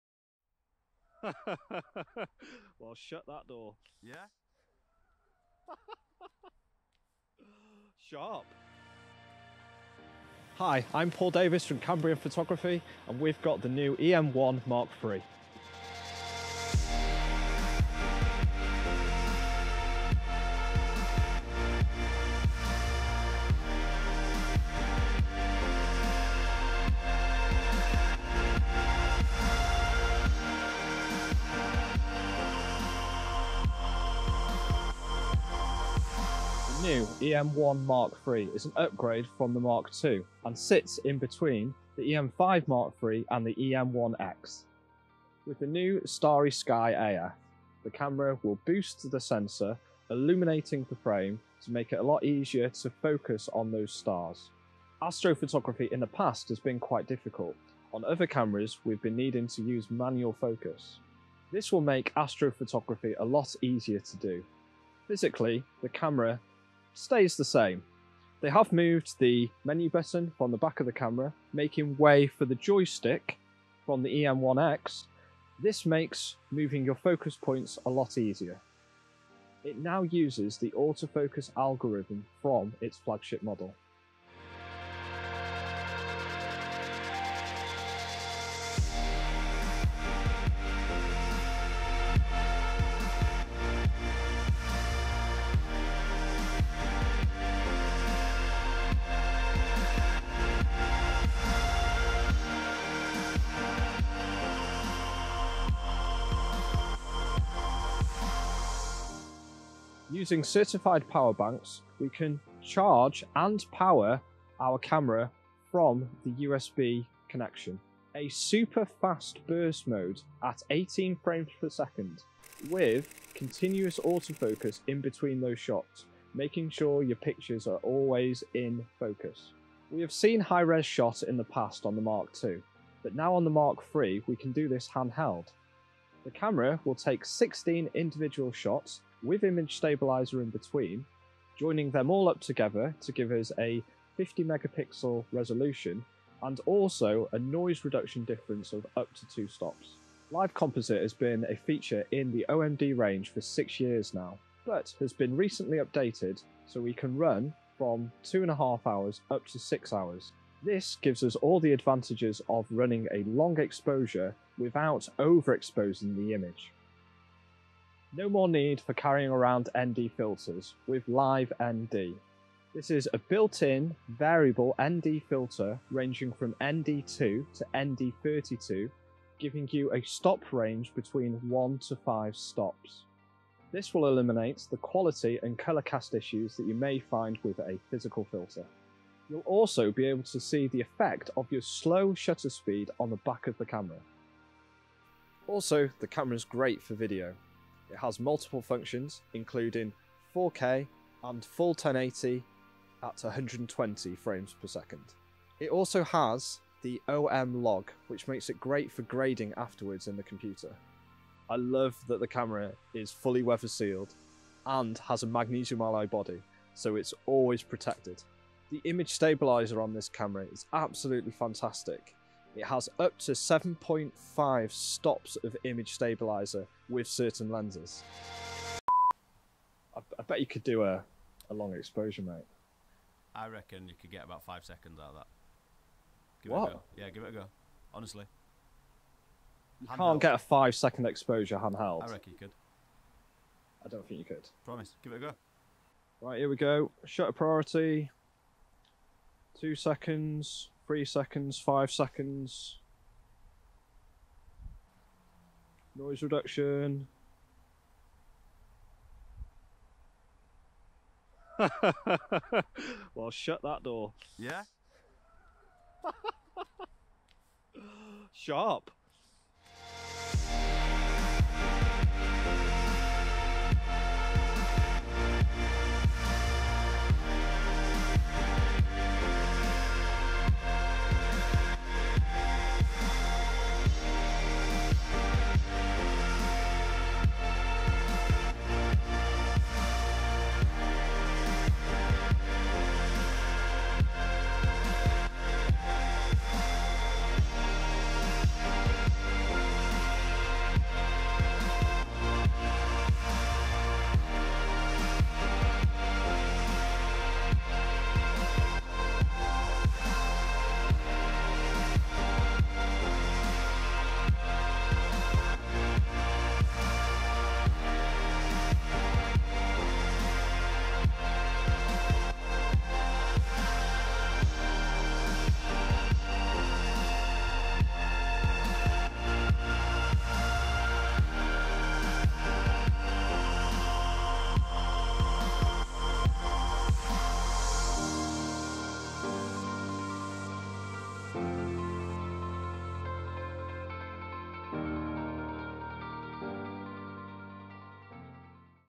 NEW Olympus O-MD E-M1iii Walk through! | Cambrian Photography | LewSpeight Visuals
Is the Olympus E-M1iii any good? Watch this video to see Paul Davies from Cambrian Photography talk us through the new features.